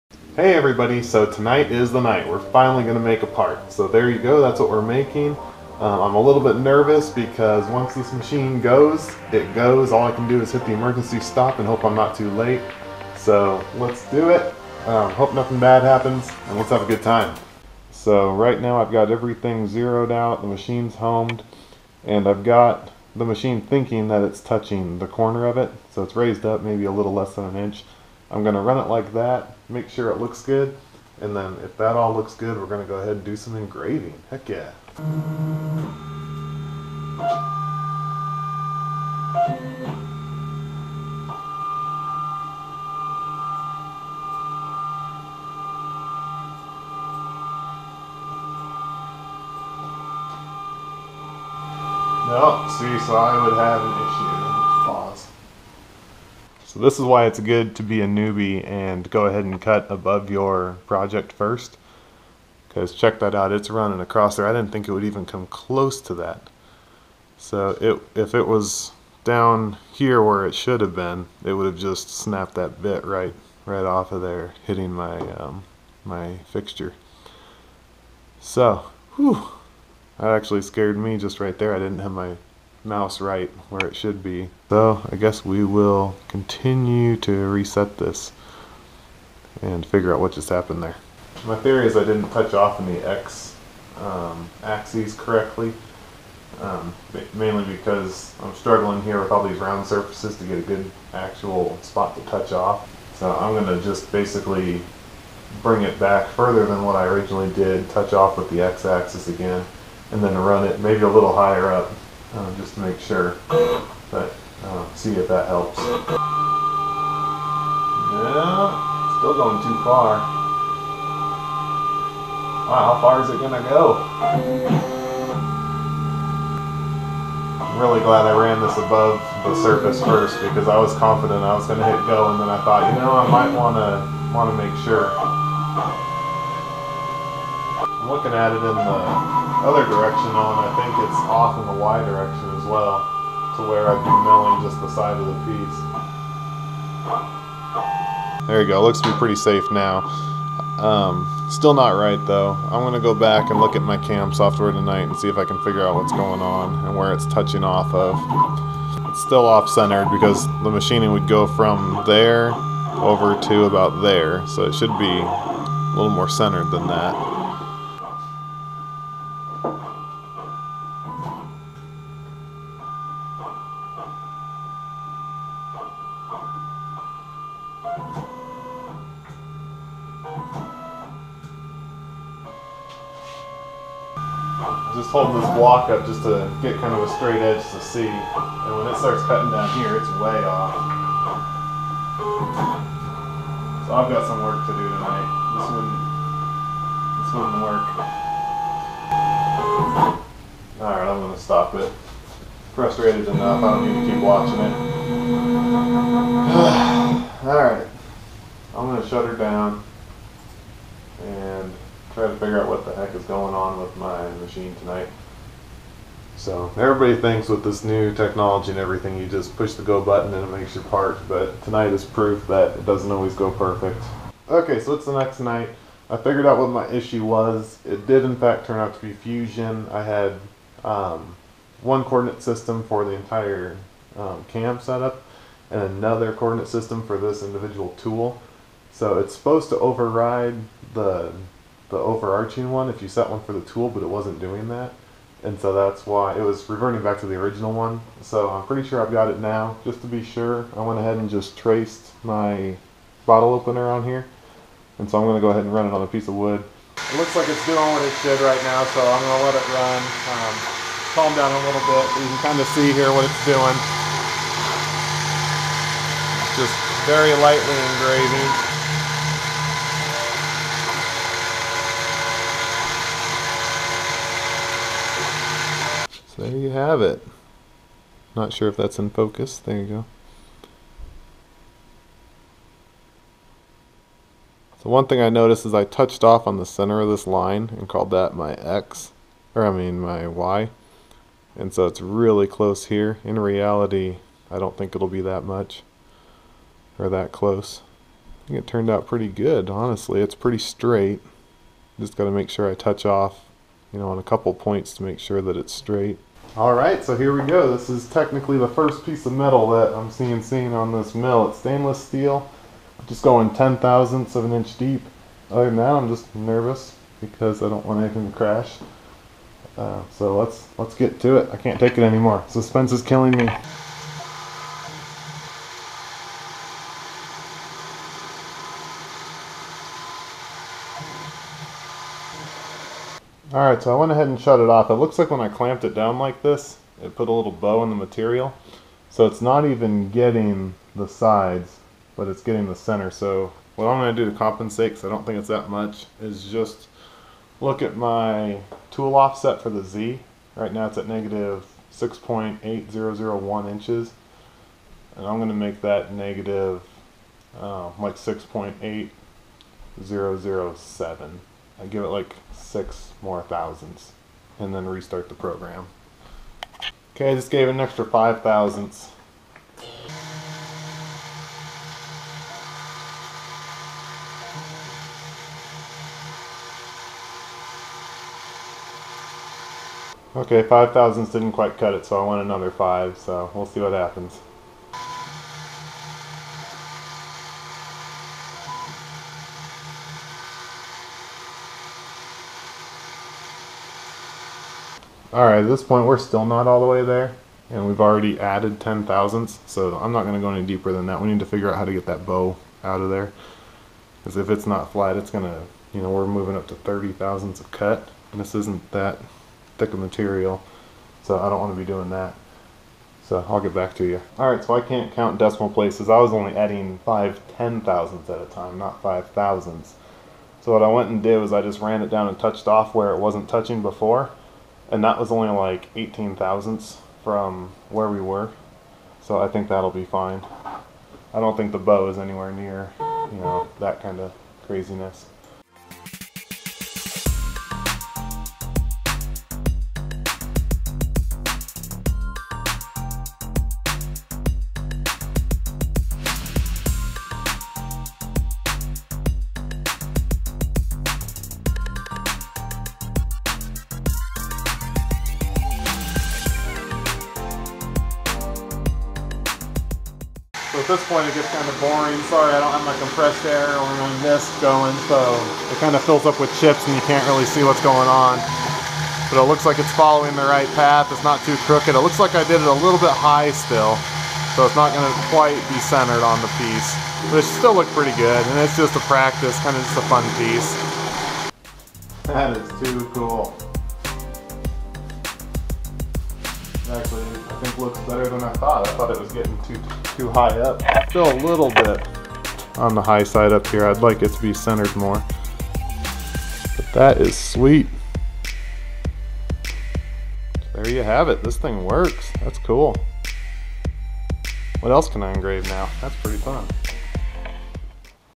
Engraving Stainless Steel Bottle Opener
I CNC'ed something!!  This is a test project to get my feet wet with the CNC Mini Mill.  I had a few struggles, but worked through them and learned a few things for the next project.  Please comment if you have any ideas of cool things to make!!